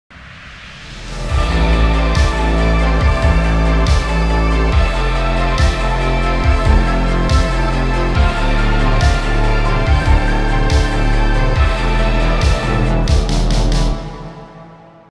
Virtual Room Singapore: Press Start - A Retro Arcade Adventure
Press Start - our 4th awesome adventure is here!

While visiting a retro video game arcade, you and your team are suddenly transported into one of the arcade machines!

Four arcade game challenges await where you are not just playing the game, you are in the game itself – experiencing old school video games using new school VR technology.

Compete with your teammates in all four arcade games until you reach the ultimate battle and try to prevent an artificial intelligence beast from invading the real world!

Can your team succeed and will you finish top of the virtual podium?